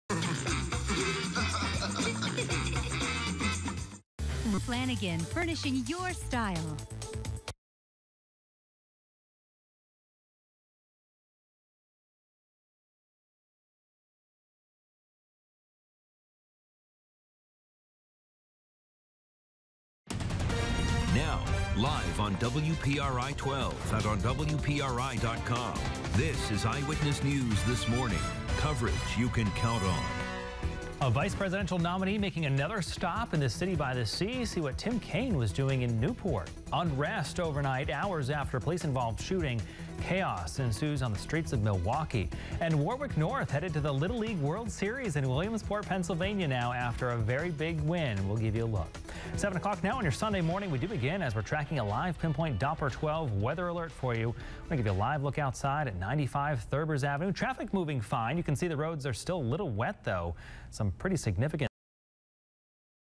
WPRI 12 Abortion Newscast
Those dastardly computers strike WPRI 12 where things are frozen and they can't start the newscast. Also this is WPRI 12 and they give the wrong  ID for WNAC Providence New Bedford (Fox Providence)   8/14/16 7am